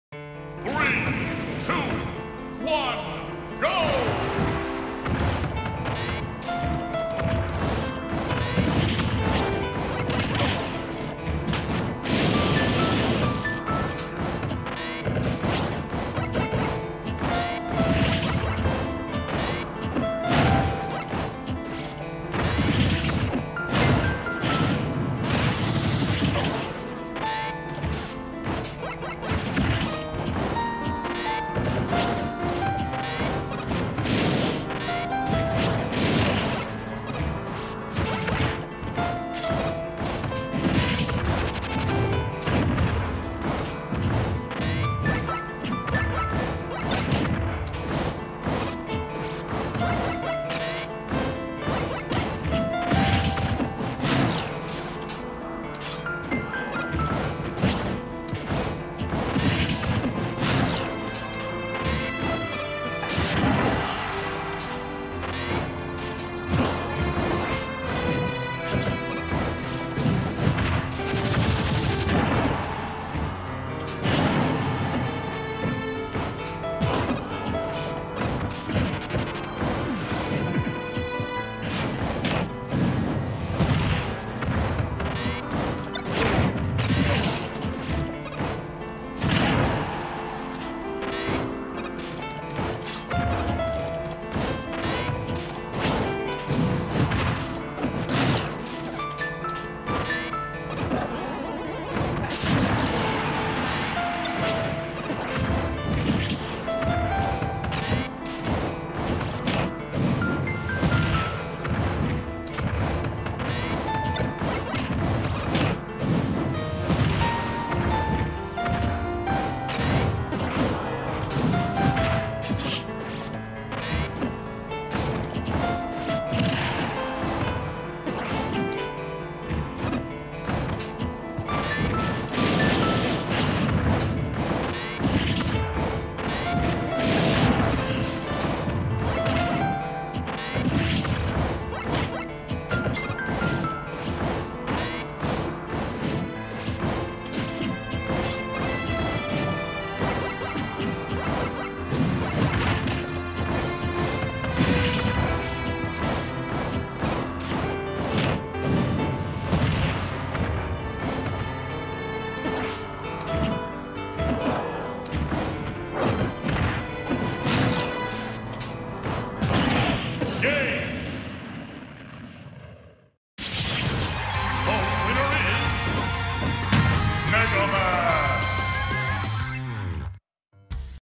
Spiked Turtle III: The Fisting - Wiggy (Mega Man) v. dorf*vader (Bowser)
The Spiked Turtle now takes on the Super Fighting Robot! I sense a similar ending to the previous two features...

Mega Man, Bowser